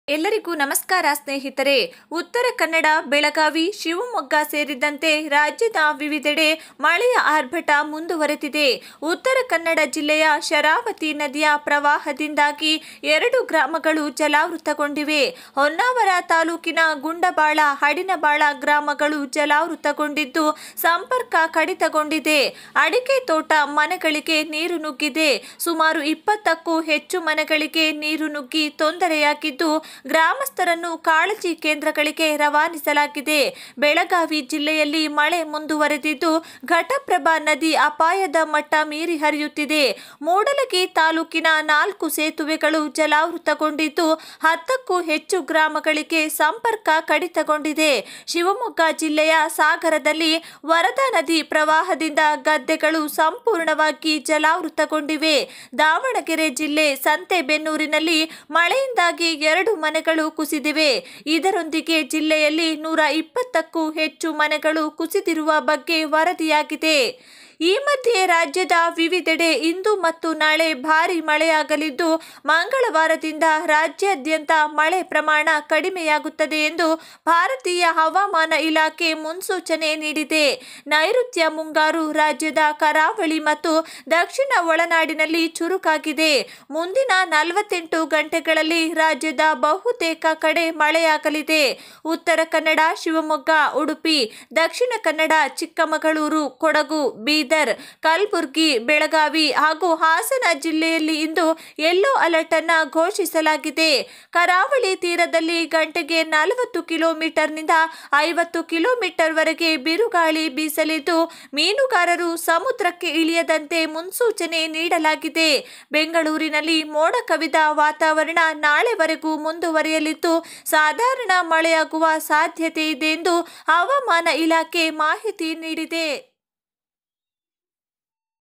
Heavy Rainfall continue in karnataka // Today karnataka weather forecast Live updates // Red alert
ಮತ್ತೊಂದು ಚಂಡ ಮಾರುತ ನಿರೀಕ್ಷೆಯಂತೆ ರಾಜ್ಯಕ್ಕೆ ಮಳೆ IMD ಎಚ್ಚರಿಕೆ ಘೋಷಣೆ ಮಾಡಿದೆ